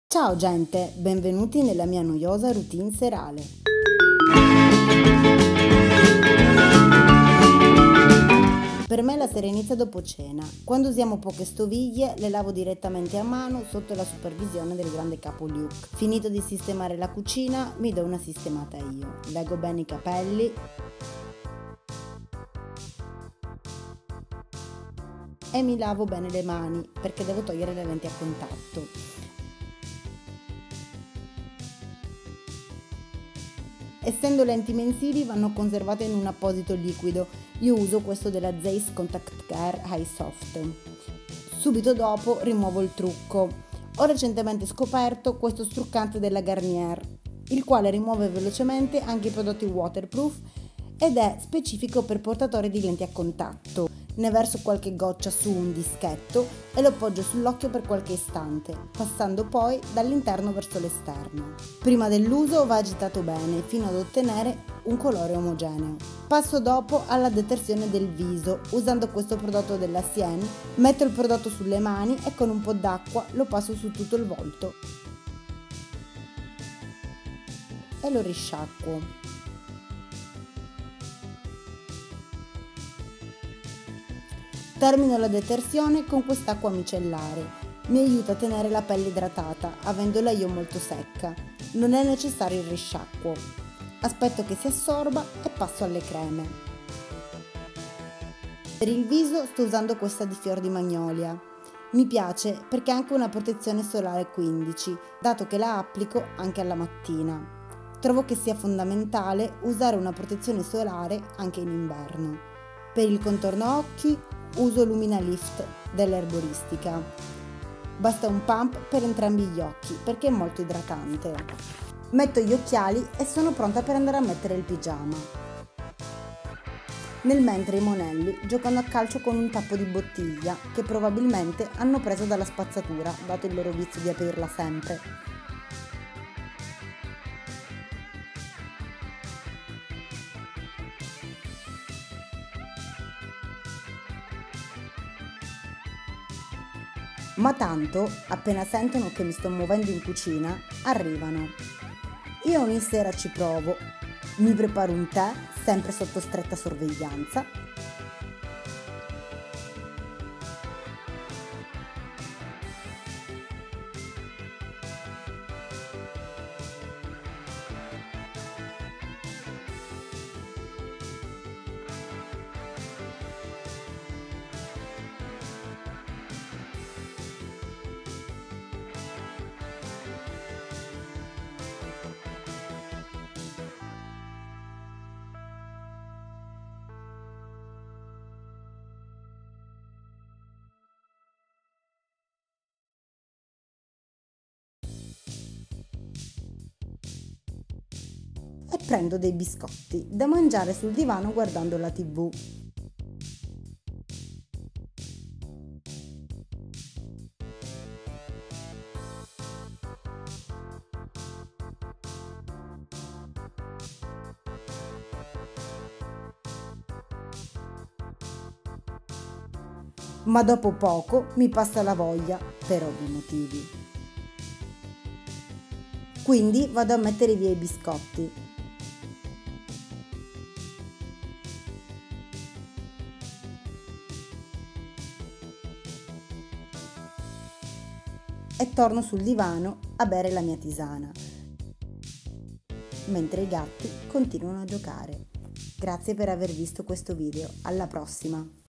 SKIN CARE ROUTINE e NIGHT ROUTINE //la vita con Luke e Leila
Ciao GENTE!
In questo video vi mostro quella che è la mia skin care. Ho la pelle molto secca, quindi se anche voi avete questo problema vi consiglio assolutamente i prodotti che uso! Sono tornata dalle ferie che ero come una tartaruga e, con questa routine, in una settimana ho risolto il problema!

non so voi, ma io sono una persona che la sera starebbe volentieri a mangiare qualcosa sul divano. Beh da quando ho Luke e Leila mi sto trovando costretta ad abbandonare questa dolce abitudine!

Fare video è una passione, non è il mio lavoro e non lo sarà mai perché ho già un lavoro che amo.

Tutti i prodotti mostrati sono stati acquistati da me.